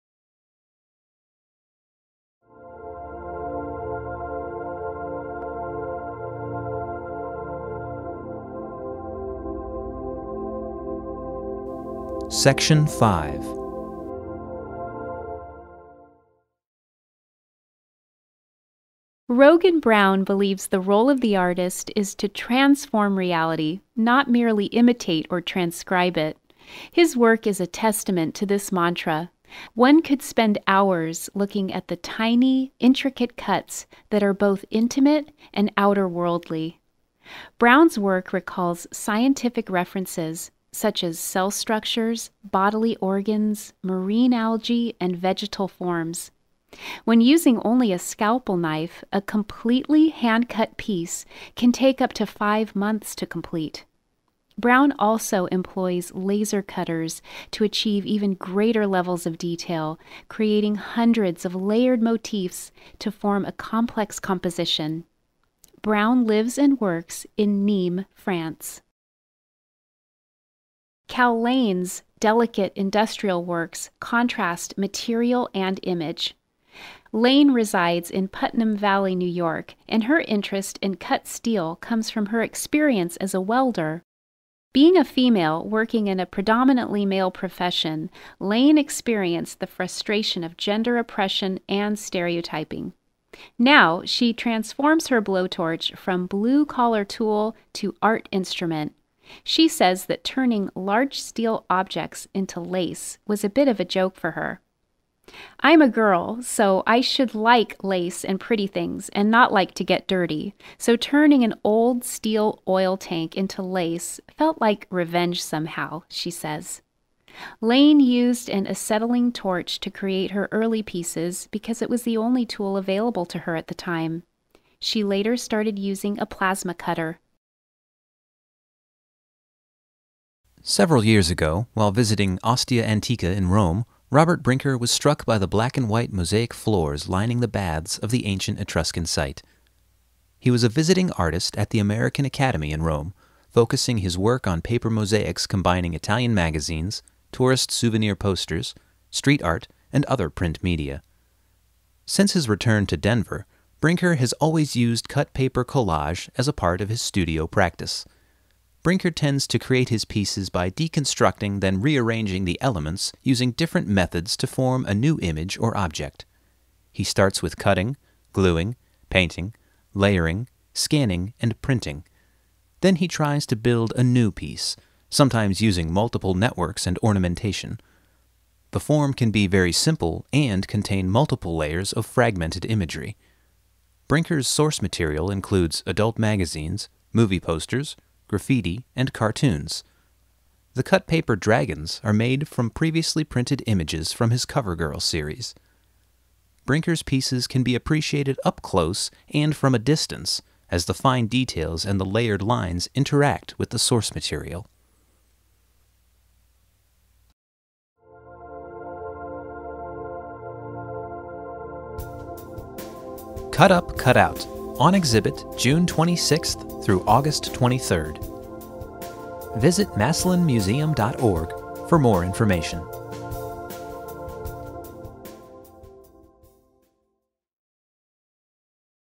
Cut Up Cut Out exhibit: Virtual Tour Section 5
On exhibit June 26 - August 23, 2020

Cut Up/Cut Out is a contemporary view of an ancient art practice that has evolved over thousands of years. This exhibition, which includes national and international artists, honors both tradition and innovation with a selection of 75 works representing diverse techniques and approaches to making art.